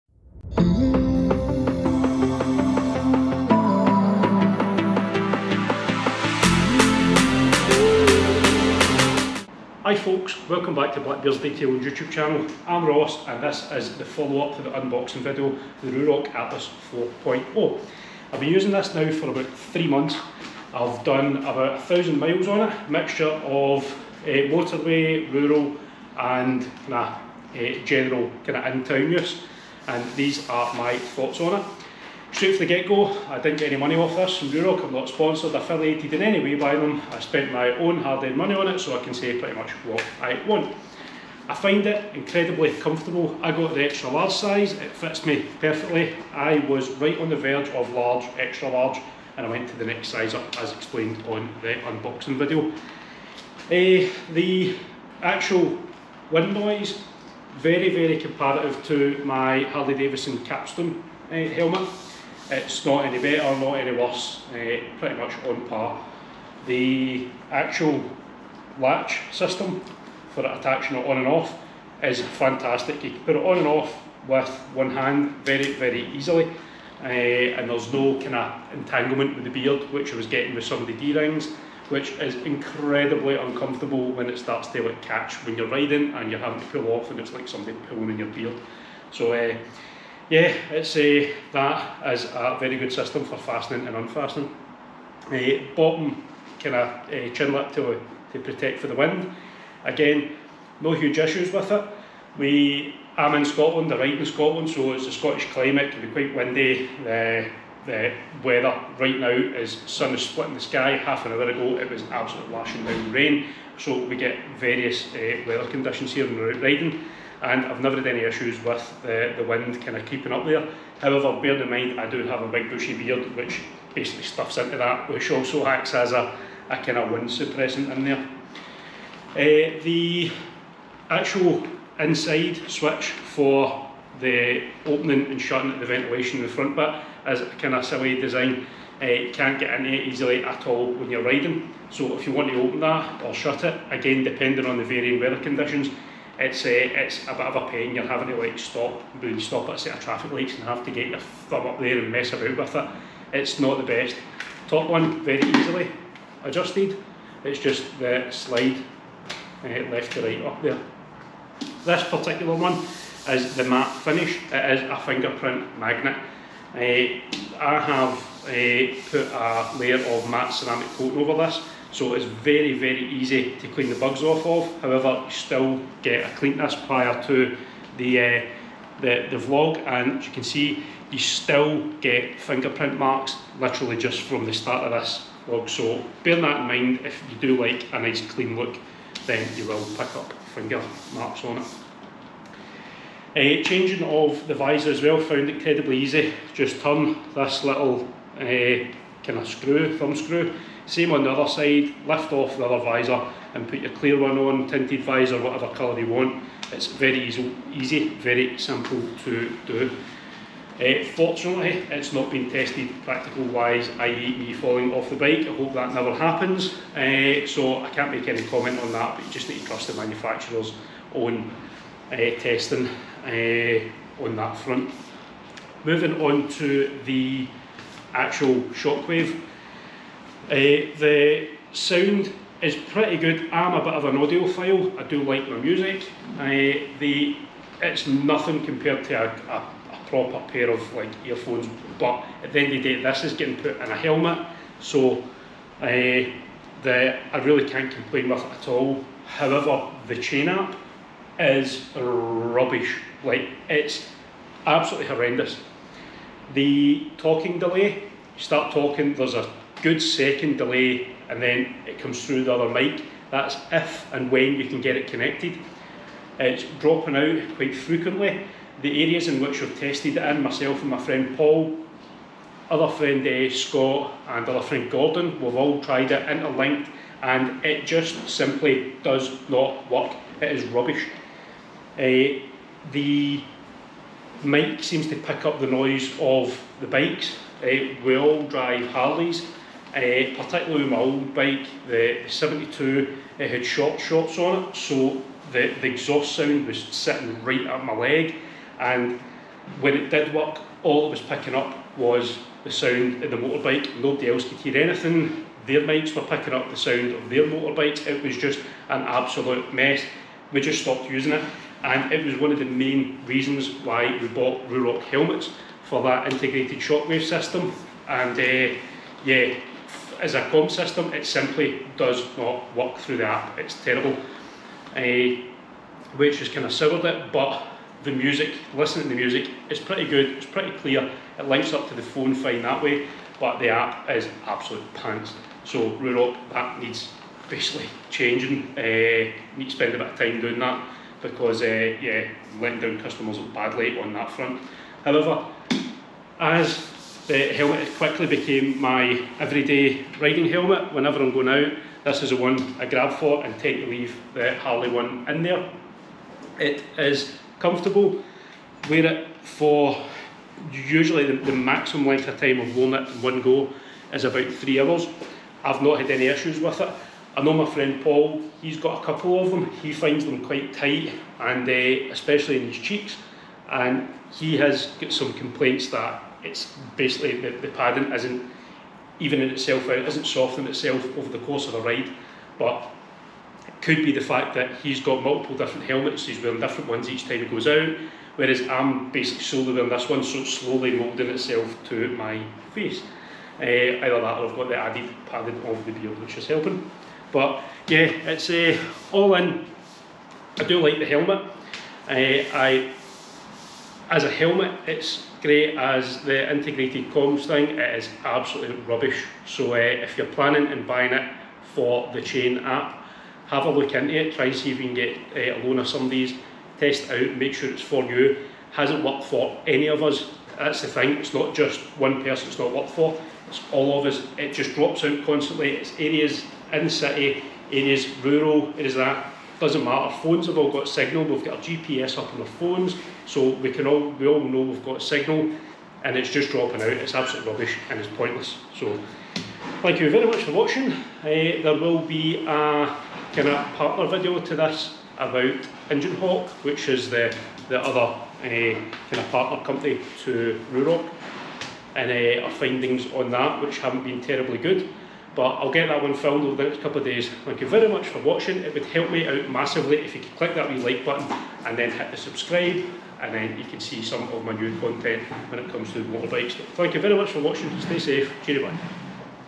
Ruroc Atlas 4.0 Core Review | Ruroc Shockwave and Chain App Review
Hi Folks,

Here is my review of my Ruroc Atlas 4.0 Core and the Shockwave 3.0 Bluetooth headset.

I have been using it solely for a number of months now and I am really impressed with it, however as you will see the Chain App does not impress me in the slightest, it is rubbish!

Let me know if you all have the same connection issues with the Chain App to help others in different regions.

Thanks,

Ross